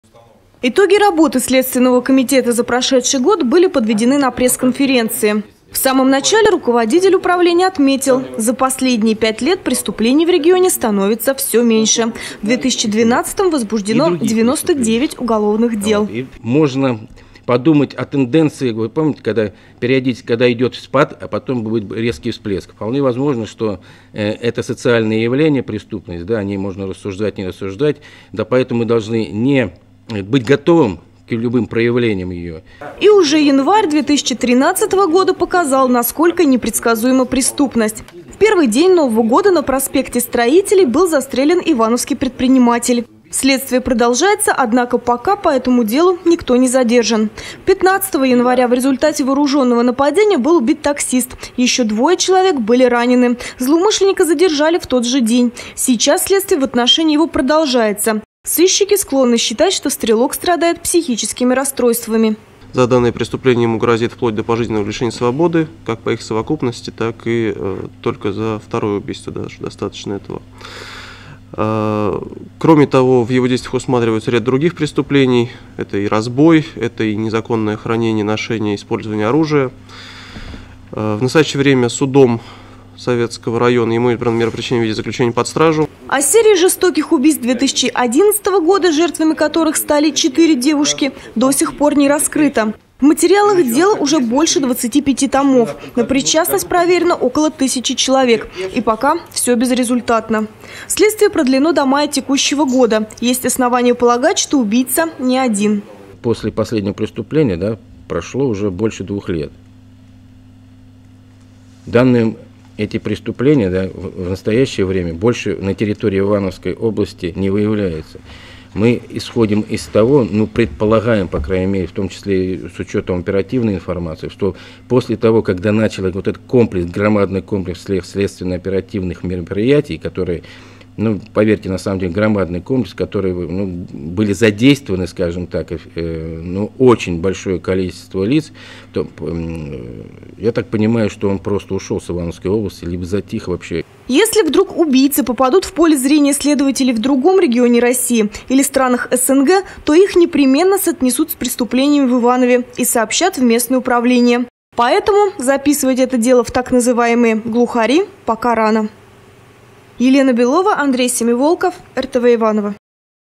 Всплеск преступной активности в начале года
Итоги работы следственного комитета за прошедший год были подведены на пресс-конференции. В самом начале руководитель управления отметил-- за последние пять лет преступлений с регионе становится все меньше.